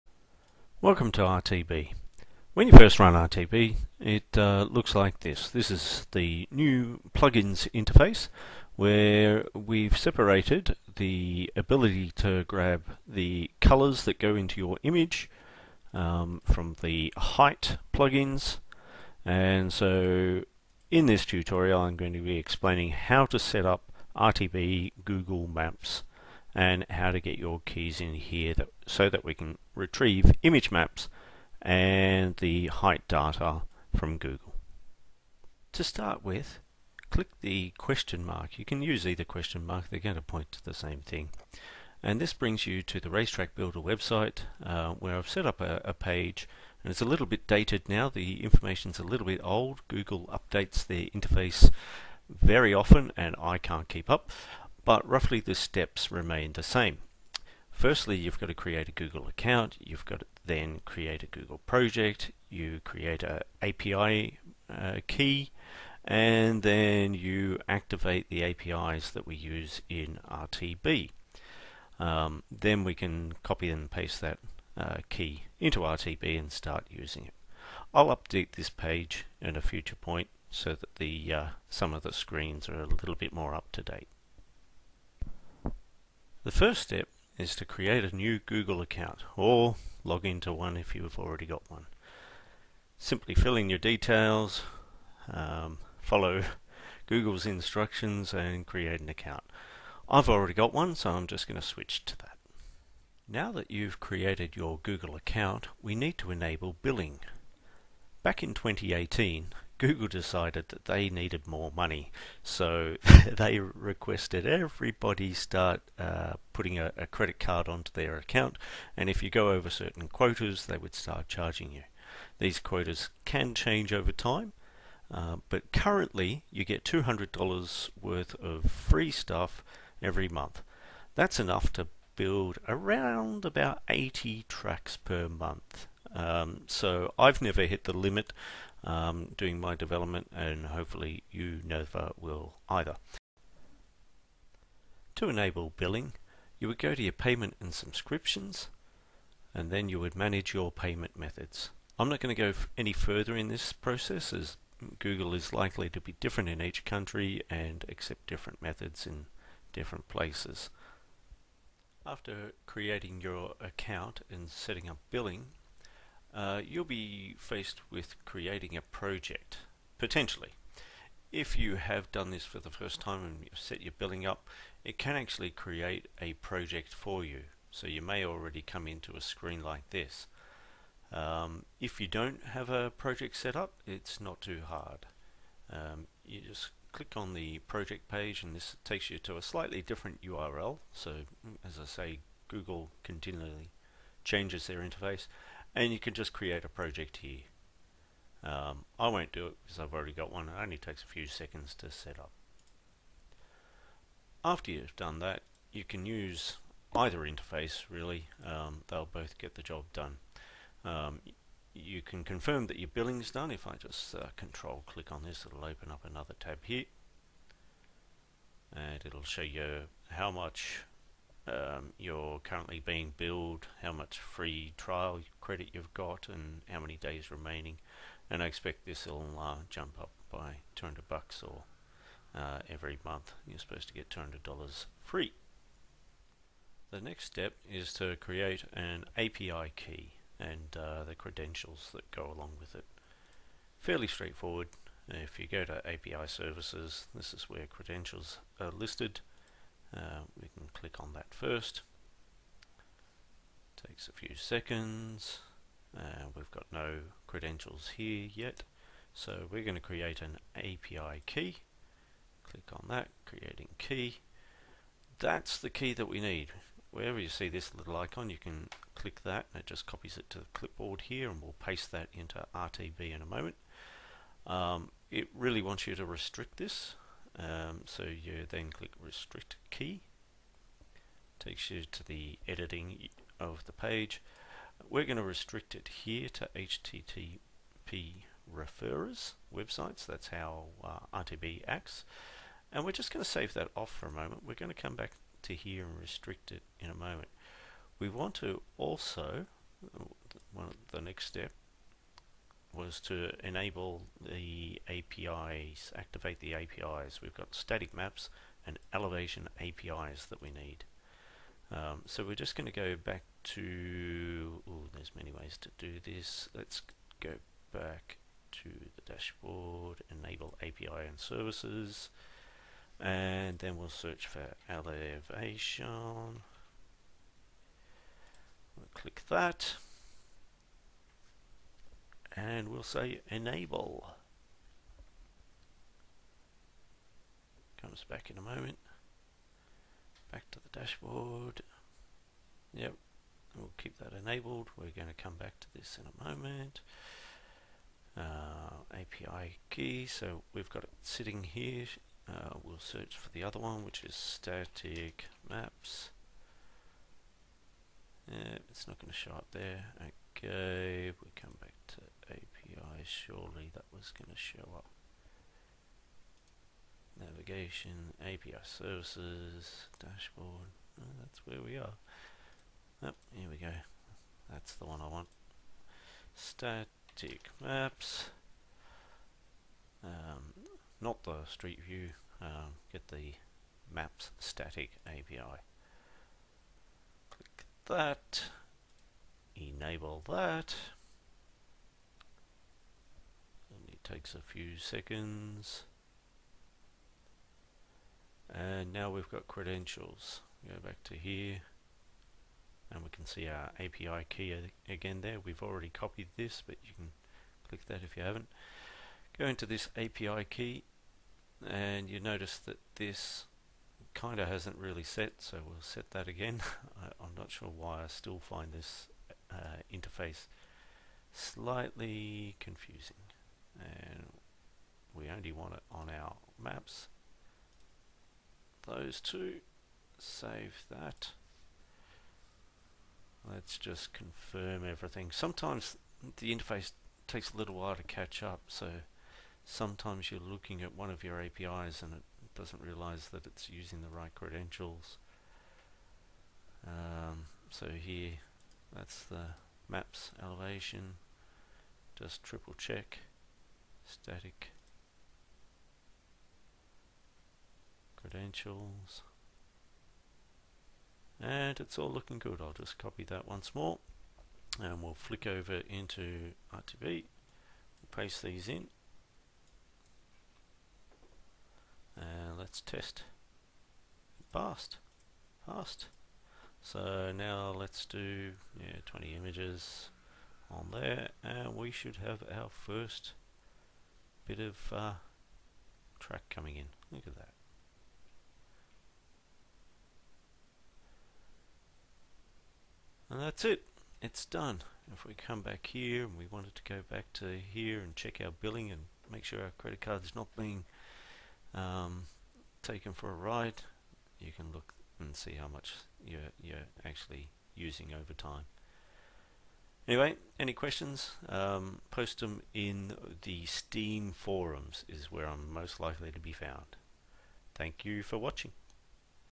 RTB Google API
How to create a Google Account, Project and API Key so you can start getting Google data into Race Track Builder.

This is for the new version currently in late stages of beta testing.

RTB Steam forum post explaining Billing.